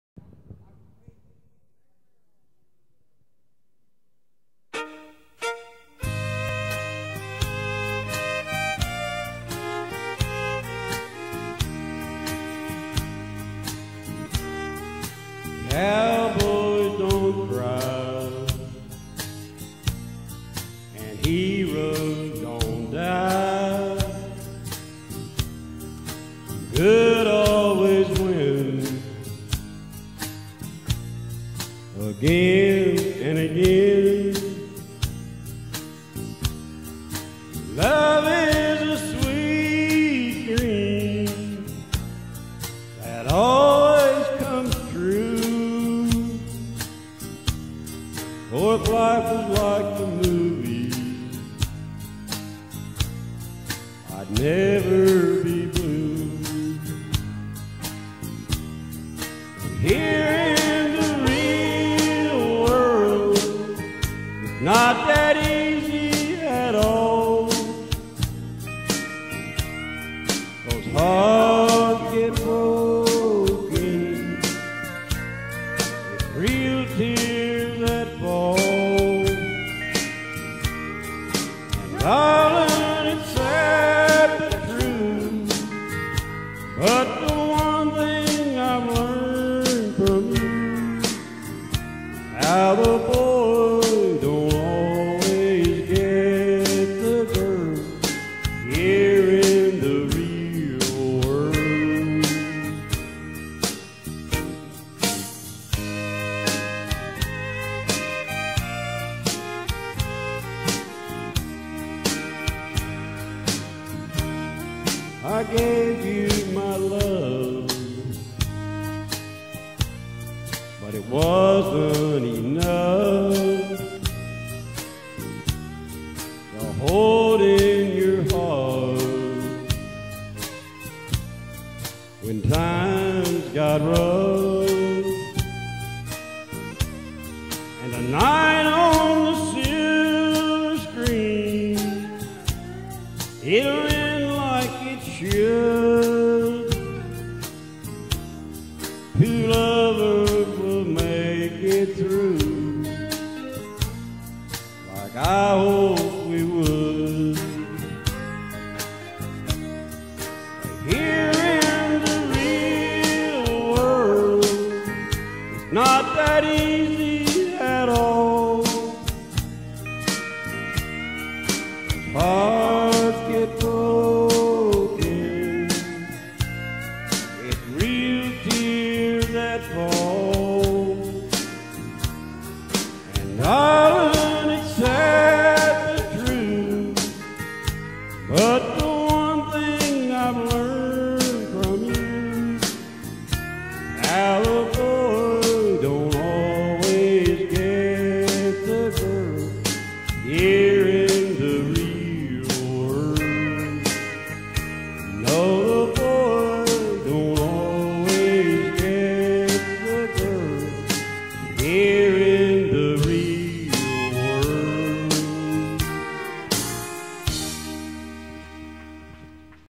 Roger - Thursday Night Karaoke - Michalo's Pizza
Covington Productions Presents Live Video From Our Thursday July 14, 2011 Karaoke Show -- Michalo's Pizza - Downtown Piqua - Come Join Us Each Week